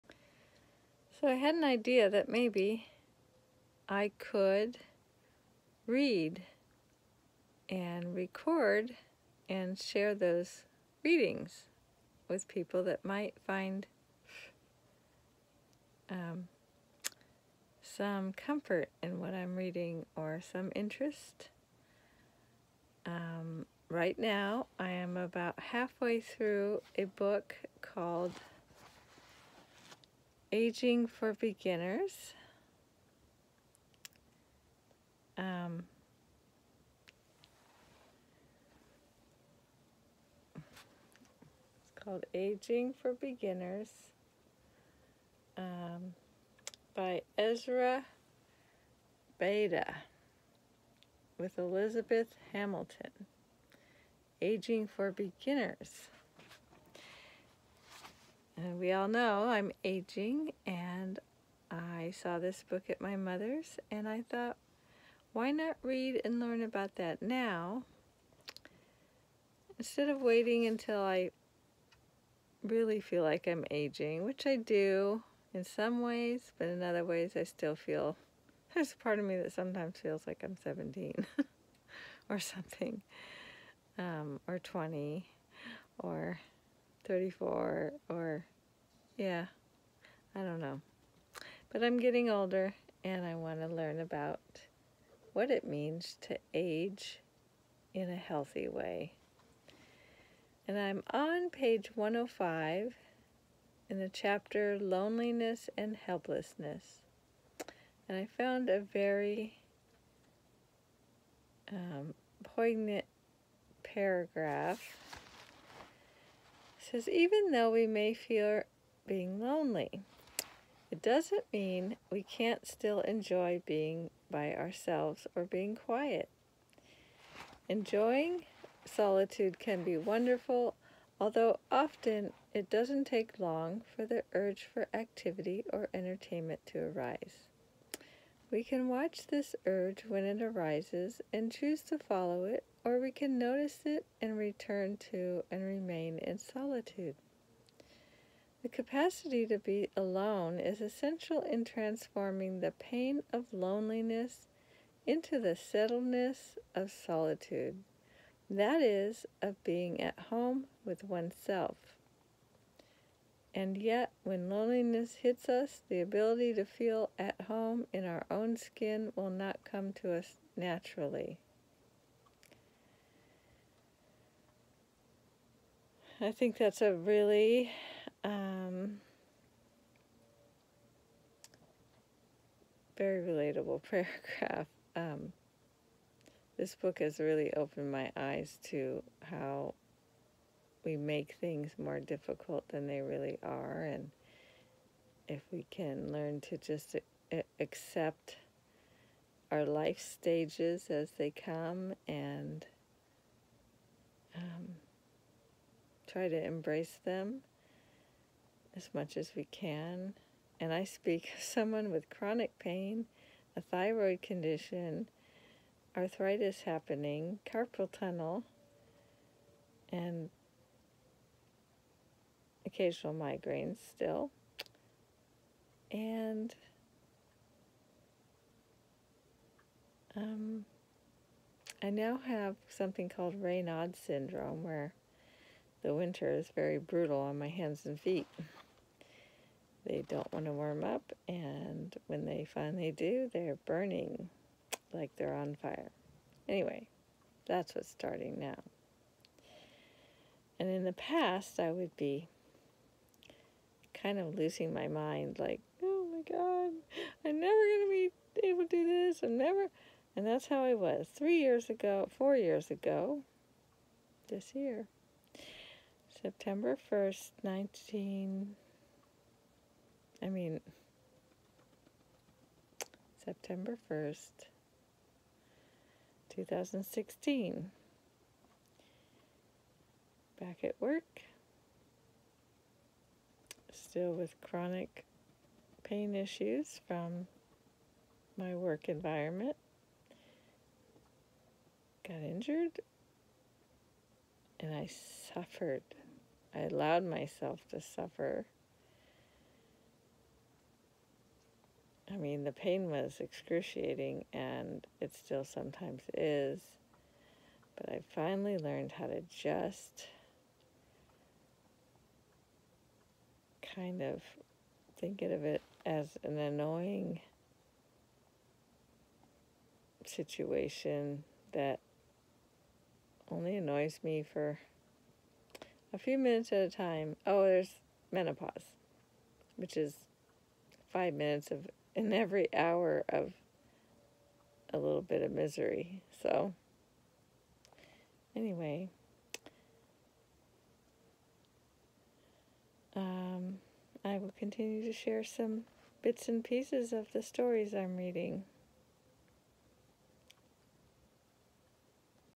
Aging Well! Reading During COVID!
During the shelter in place…the first leg of the pandemic. This is something I thought I was going to continue to do, but I didn’t continue. I guess I found more interesting things to do during my lockdown. Anyway, I hope somebody enjoys this little bit. I did enjoy the whole book and it did bring me a lot of comfort and insight as to how to age in a more acceptable way. In other words how to accept my aging in a better way.￼